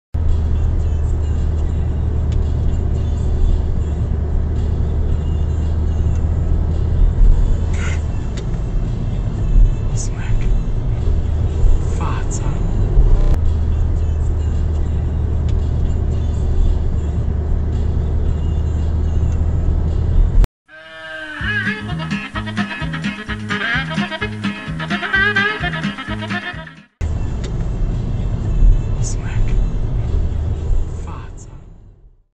Leves... majdnem! :)
Majdnem a fazékba futott a jószág, de még éppen elkerülte a szomorú, de annál ízletesebb véget! :)